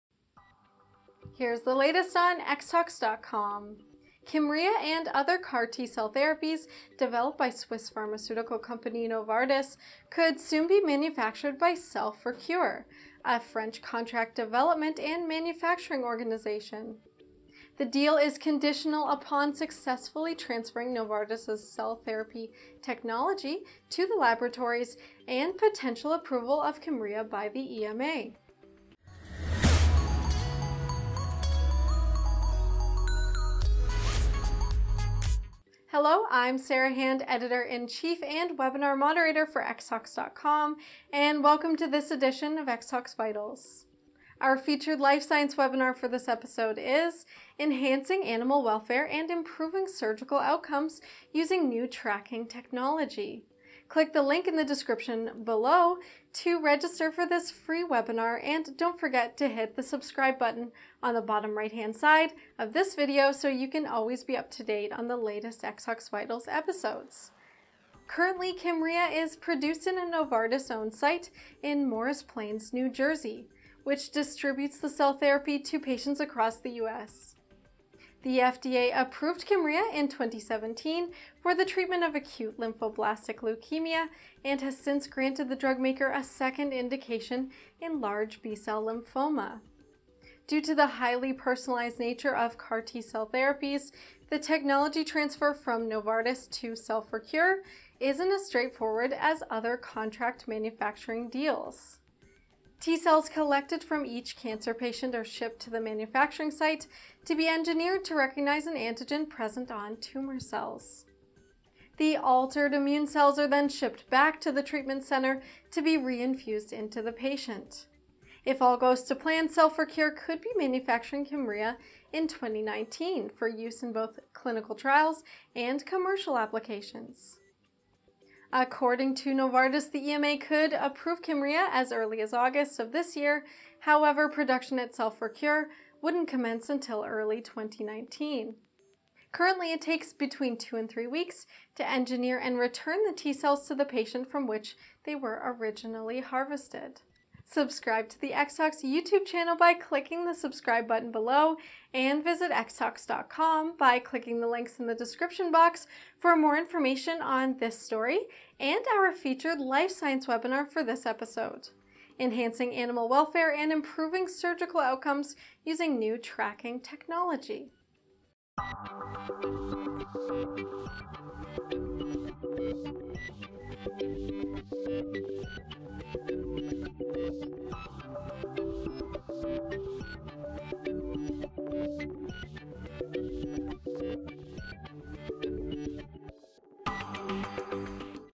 Novartis’ CAR-T Therapy Kymriah Could Be Manufactured by French CDMO Cell for Cure
Kymriah and other CAR-T cell therapies developed by Swiss pharmaceutical company Novartis could soon be manufactured by Cell for Cure, a French contract development and manufacturing organization (CDMO). The deal is conditional upon successfully transferring Novartis’ cell therapy technology to the laboratories and potential approval of Kymriah by the EMA.

Listen live and interact with featured speakers on our upcoming Life Science webinars